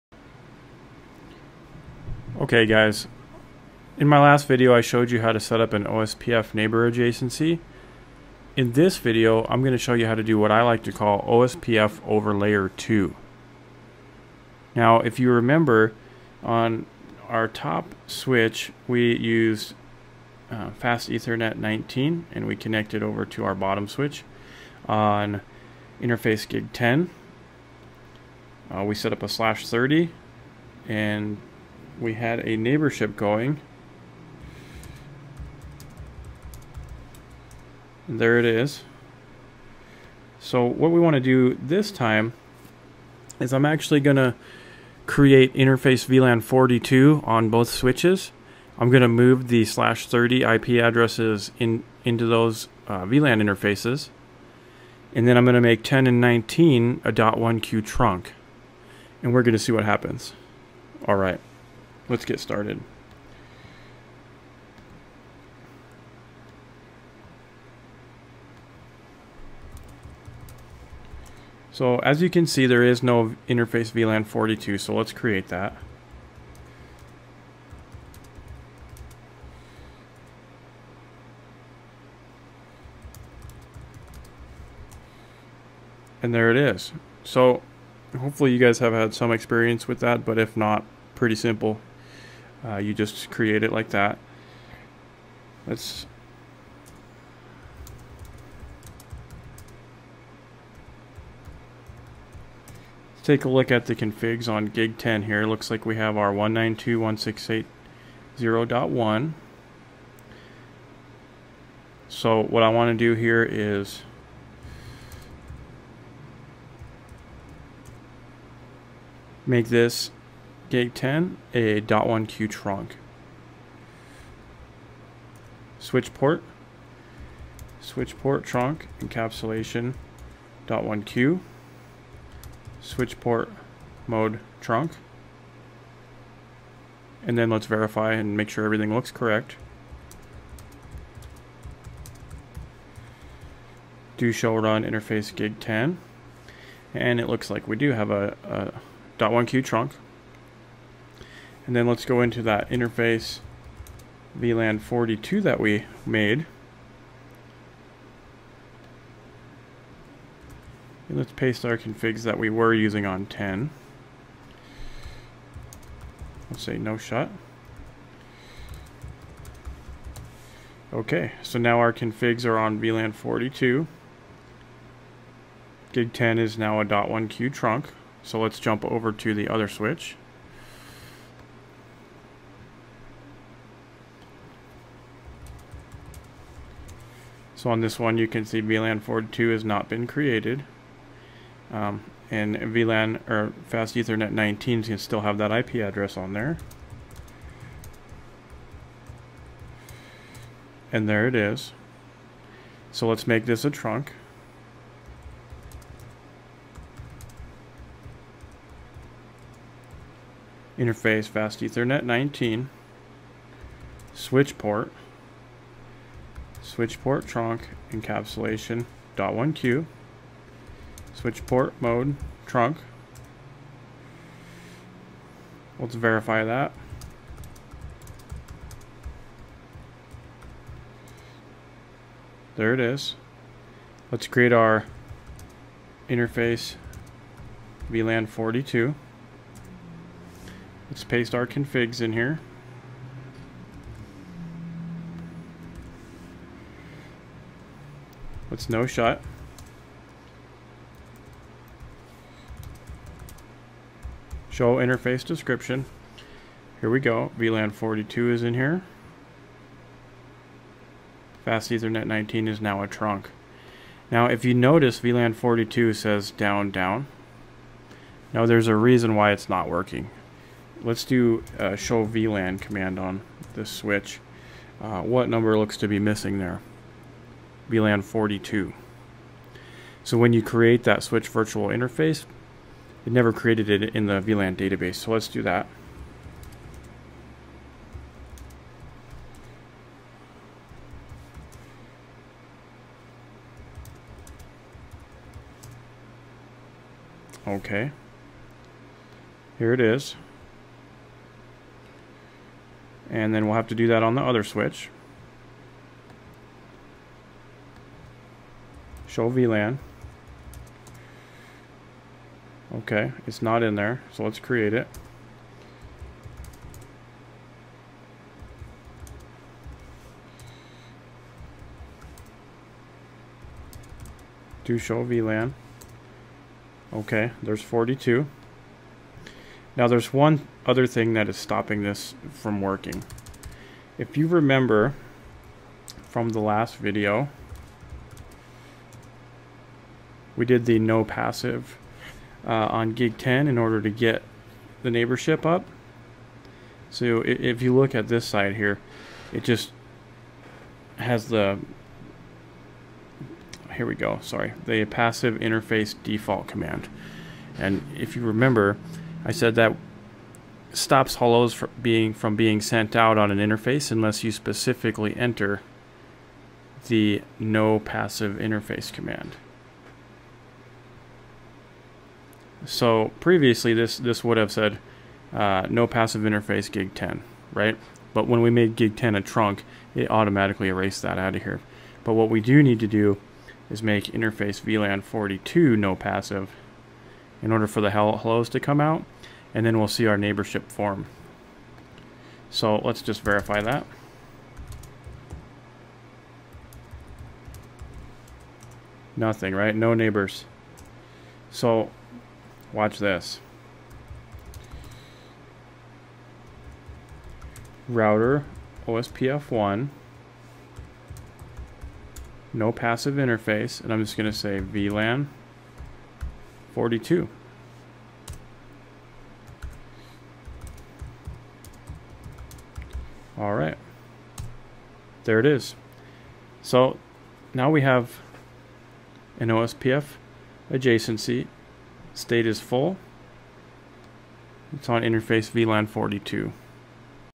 OSPF over Layer 2
In this video I show you how to set up OSPF over trunks.